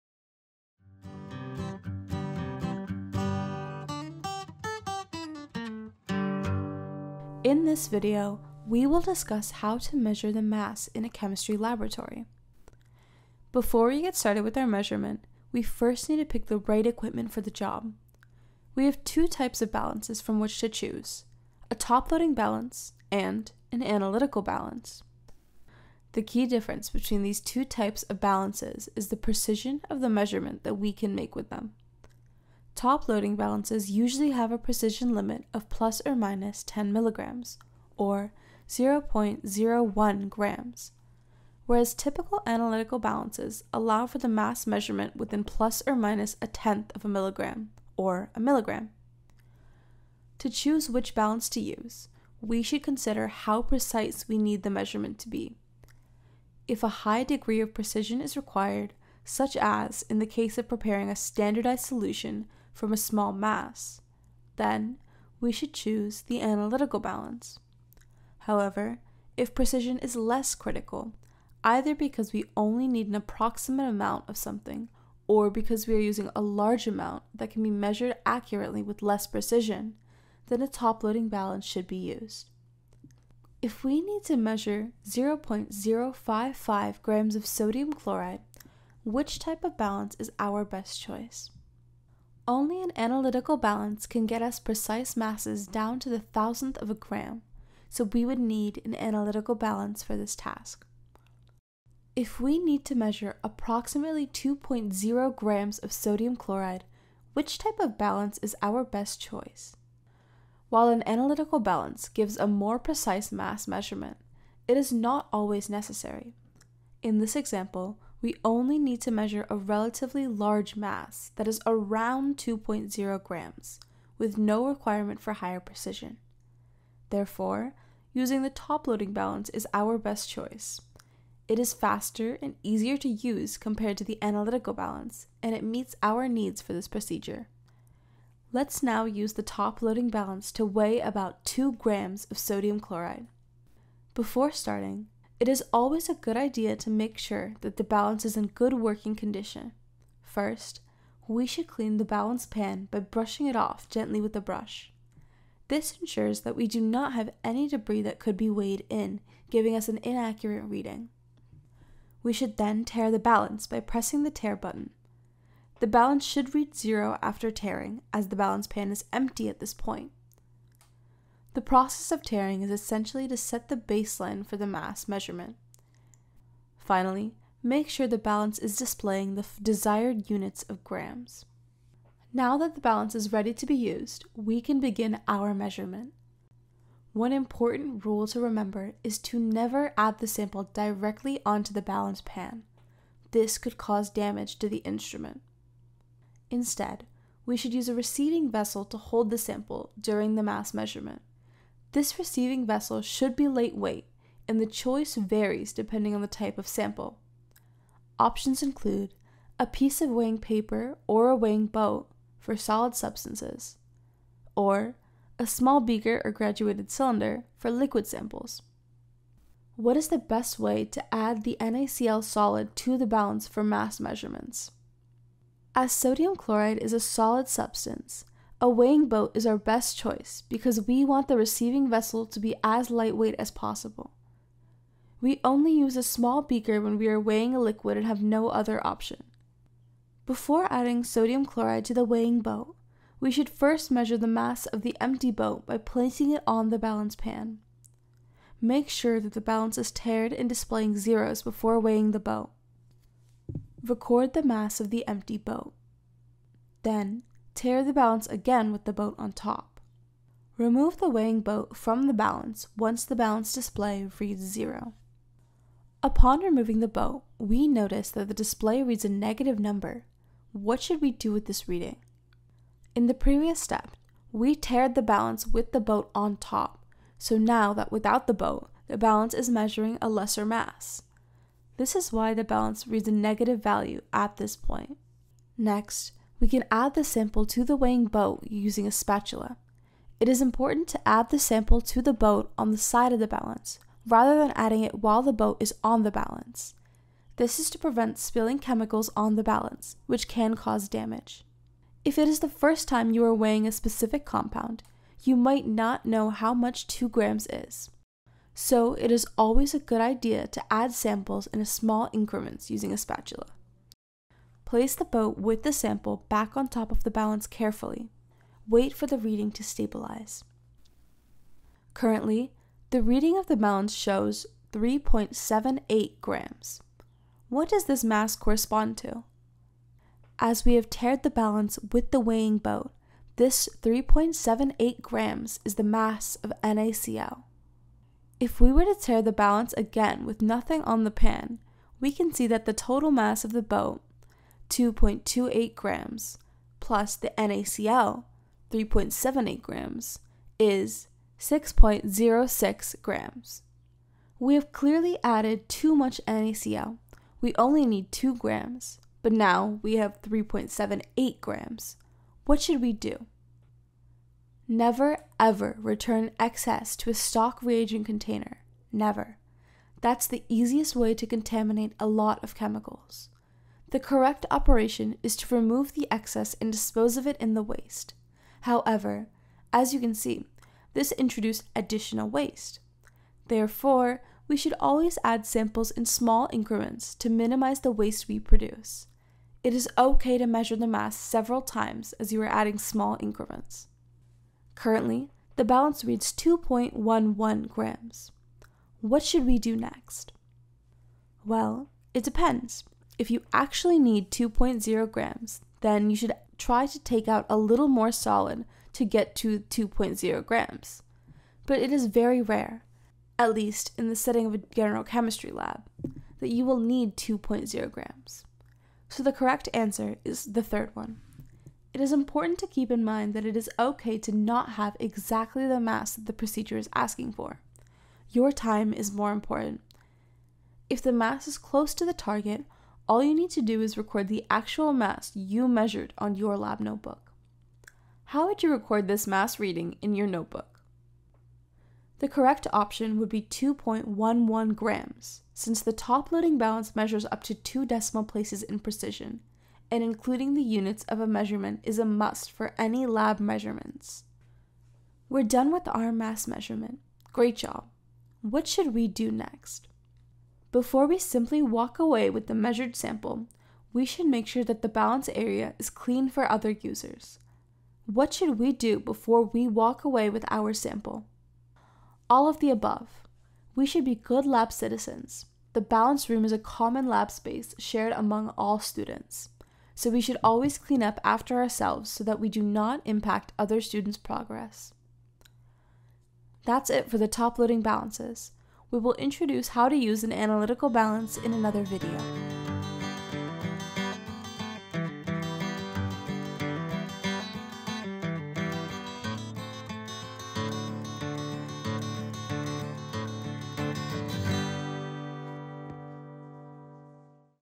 Top Loading Balance
In this video, we will discuss how to correctly measure the mass of a solid sample using a top-loading balance.

Script: Karim Saad, Meghna Patel
Audio + Onscreen: Meghna Patel
Editing: Meghna Patel, Ethan Chong
Project Directors: Keying Chen, Binyomin Abrams

This video was made possible through a grant from the Boston University Shipley Center for Digital Learning and Innovation